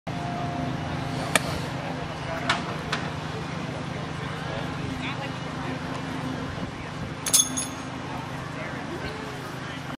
Blueberry Festival Lexington Ohio 2009 Scott Walock Heavy Weight for Distance
Scott 56# for distance